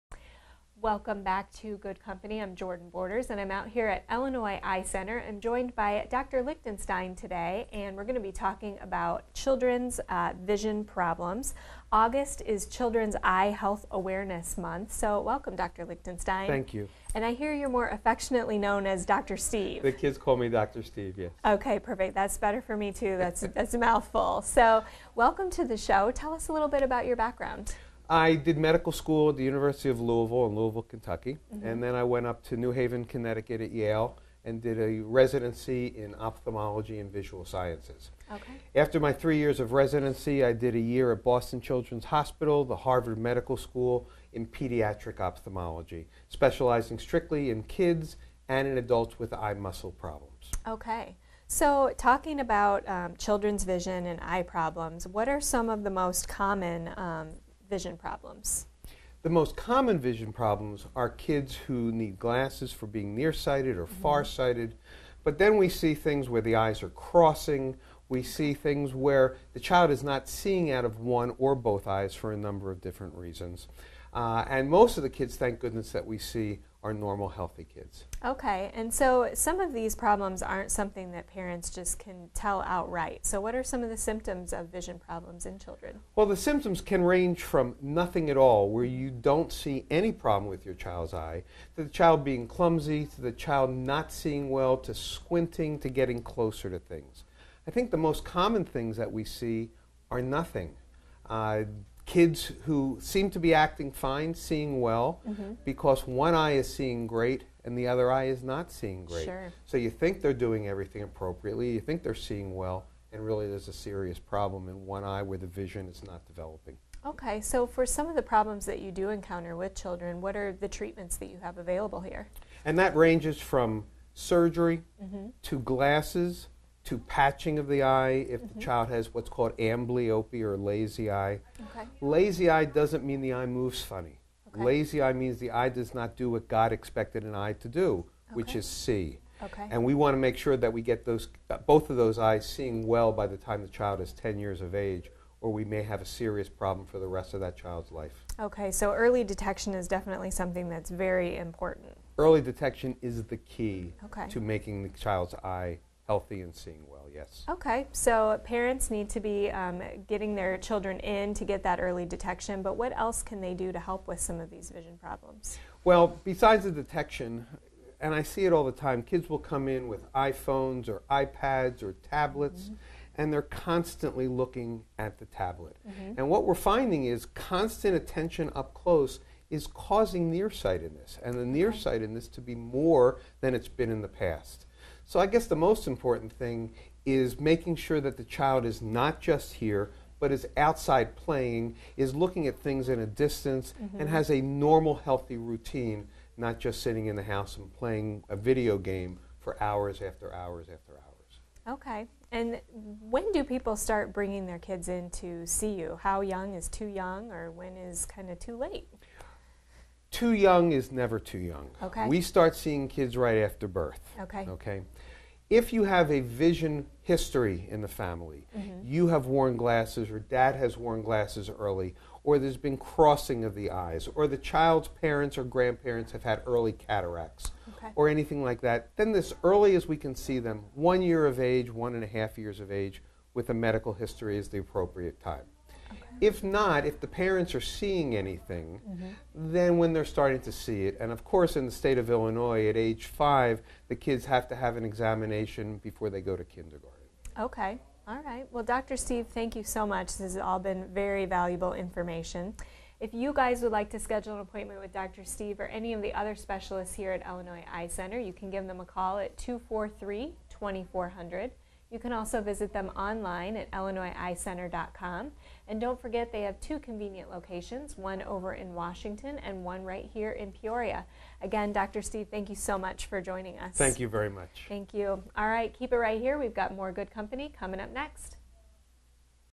Dr. Steven Lichtenstein - Pediatric Ophthalmology
Illinois Eye Center's Dr. Steven Lichtenstein talks about Pediatric Ophthalmology during his recent appearance on WHOI's Good Company.